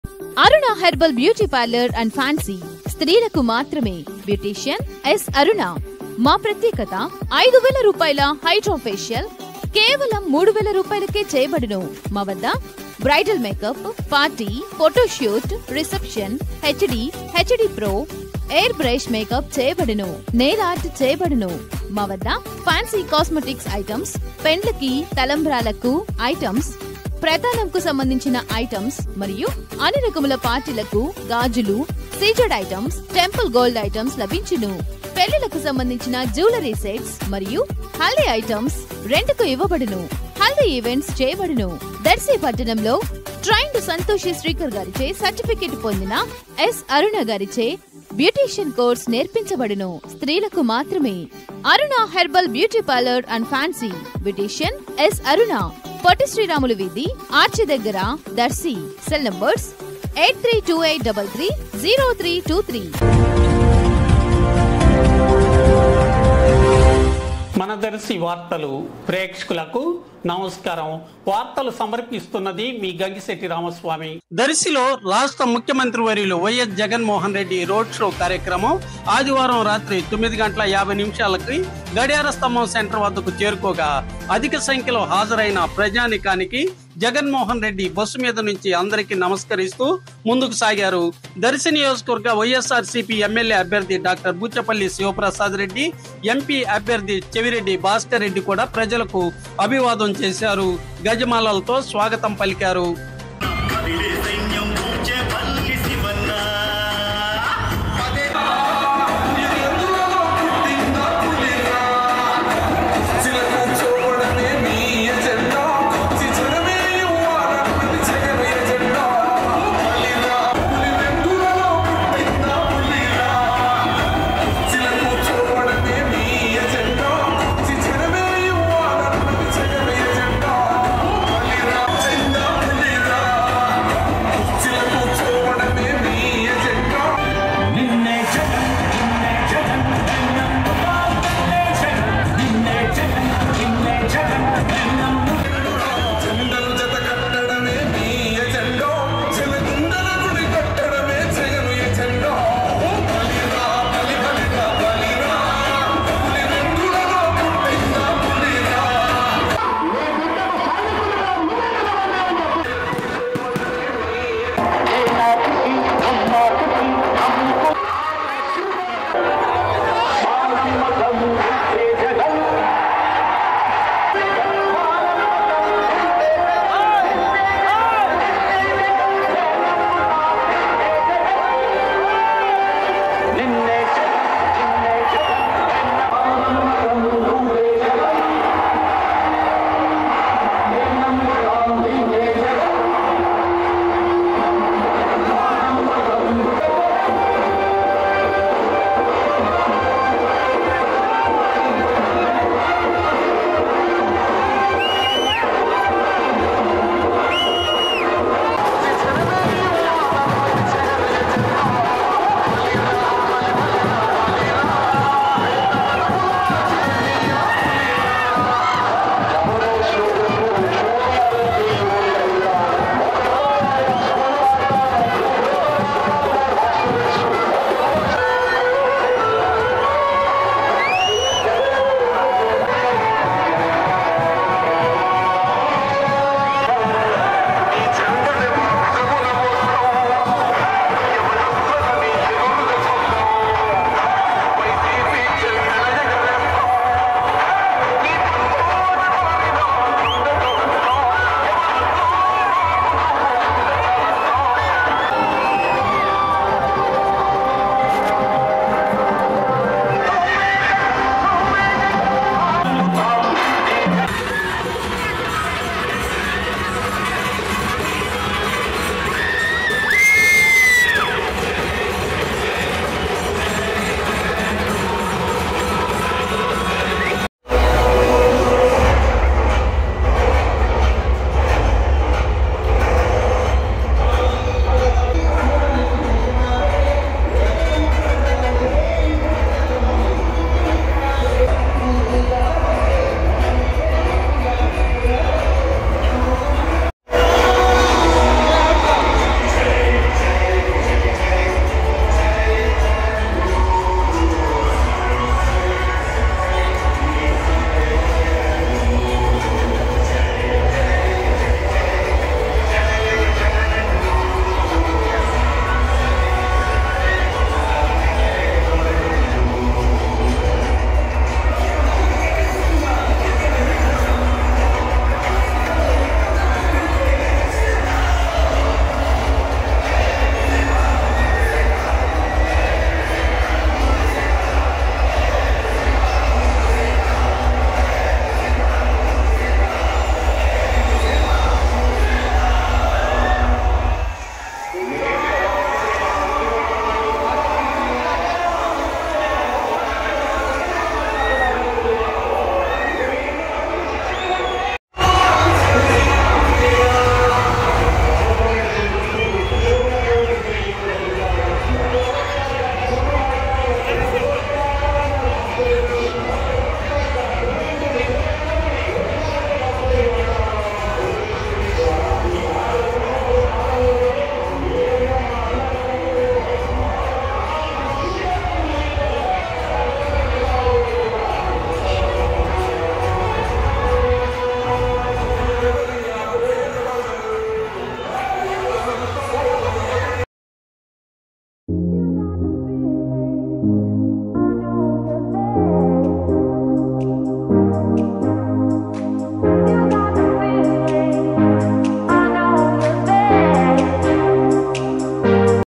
జగన్ రాకతో జనసంద్రమైన దర్శి Cm Ys Jagan Program In Darsi | Mana Darsi News
Cm Ys Jagan Program In Darsi, Prakasam Dt.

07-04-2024
Reported By
Gangisetty Sai Teja,
OutPut Editor,
Mana Darsi News

Reg. Office:-
DARSI NEWS
DARSI NEWS UPDATES
DARSI VAARTHALU